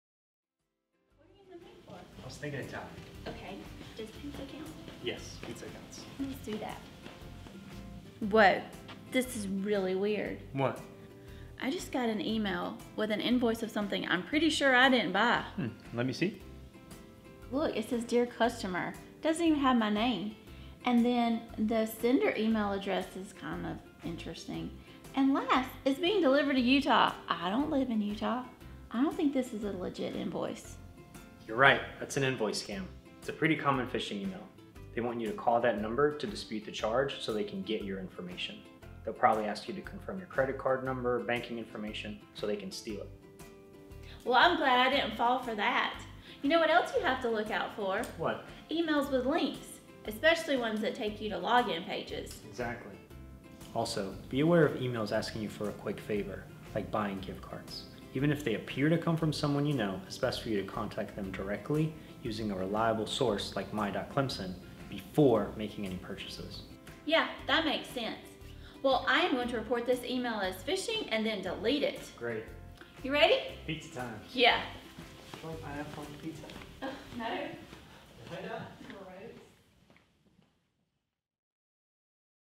Phishing Email
Learn what to look for to help avoid phishing emails.